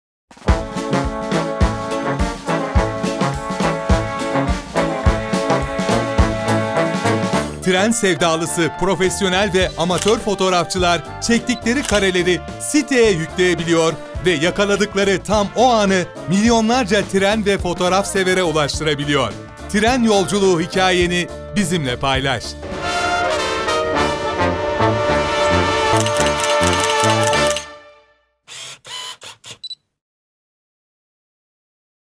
TAM O 'AN' DOĞU EKSPRESİ TANITIM FİLMİ
KUKA Yapım tarafından hazırlanan Tam O An internet sitesinin tanıtım çalışması

Tren ve trene dair çektiğiniz her kareyi bizimle paylaşabilirsiniz